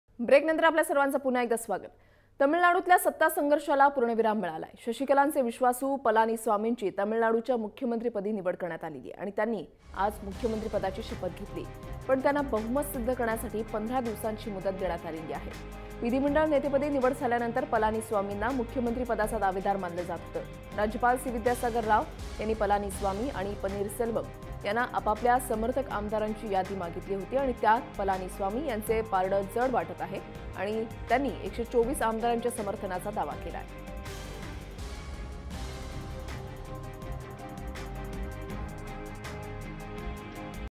Edappadi Palaniswami takes oath as 13th Chief Minister of Tamil Nadu
Edappadi K Palaniswami was sworn in as the 13th Chief Minister of Tamil Nadu on Thursday by Governor C Vidyasagar Rao.
Palaniswami took oath first, followed by 30 ministers in four batches. The swearing in ceremony was held at Raj Bhavan around 4.30pm.
This is the third ministry in the last 10 months since AIADMK won the assembly election in May 2016.
Almost simultaneously, police protection given to O Panneerselvam at his official residence was withdrawn. The chief minister's emblem and beacon on his car (TN 04 AX 2345) were removed.